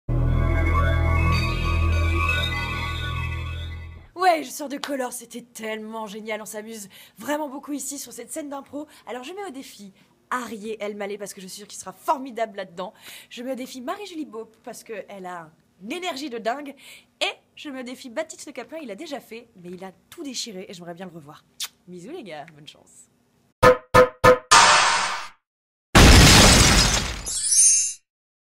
Colors challenge - Dounia Coesens
Après ses passages dans COLORS en impro totale, Dounia Coesens met au défi ses amis comédiens et humoristes... Qui sera capable de relever le défi ?